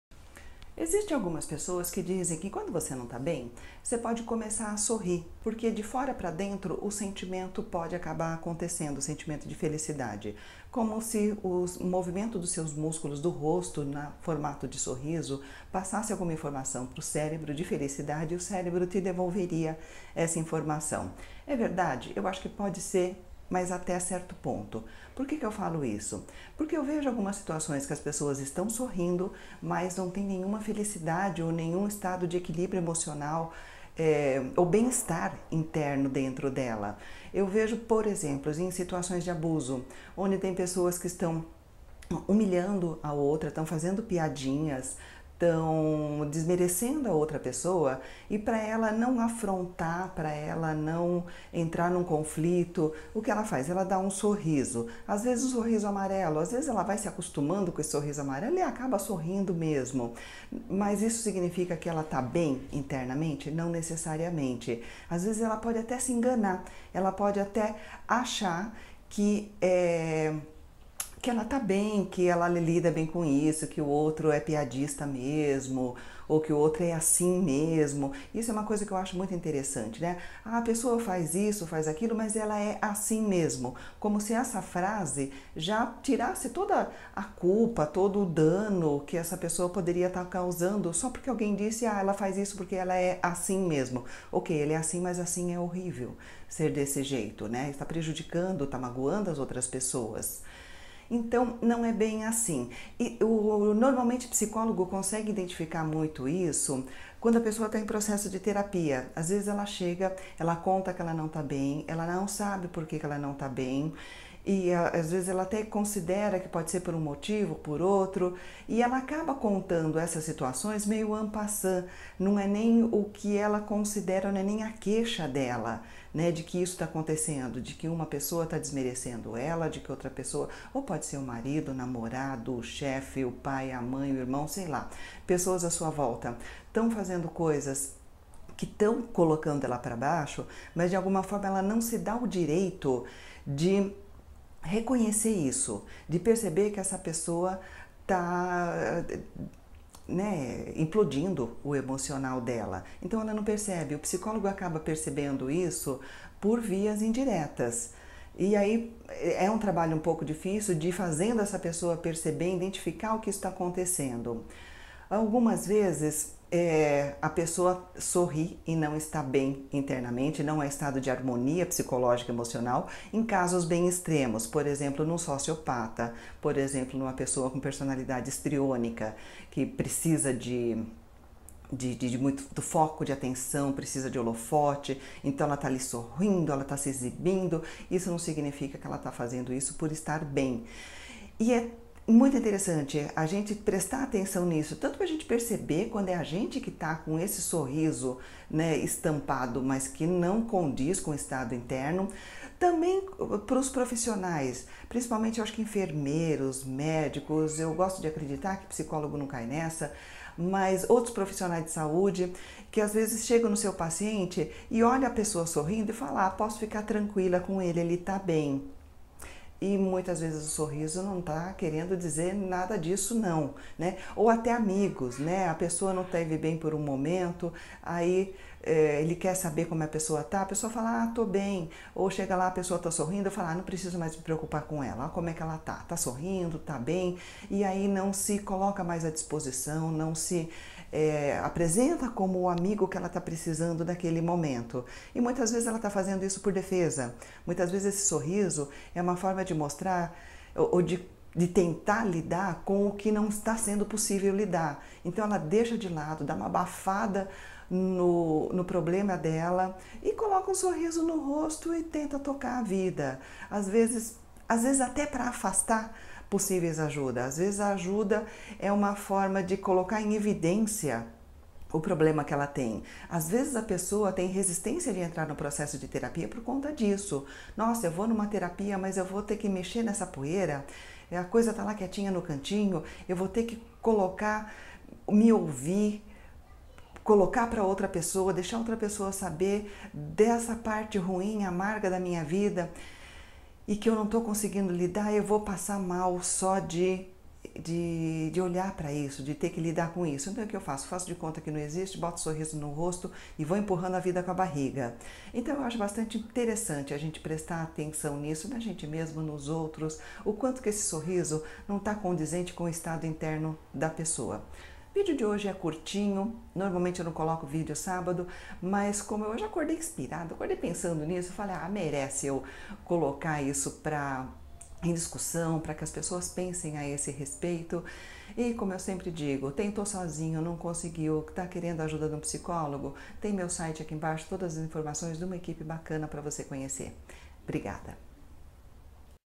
Um sorriso não prova ausencia de sofrimento
Nem sempre sorrimos por felicidade, algumas vezes estamos tentando afastar um desconforto, outra demostra dificuldade em lidar com abusos, outras vezes pode fazer parte de um quadro patológico.

Rua Bela Cintra, 968 (prox Av Paulista -  Metrôs Consolação e Paulista)
Rua Frei Caneca, 33 (Consolação, prox ao centro)

Marisa de Abreu Alves
Psicóloga CRP 06/29493-5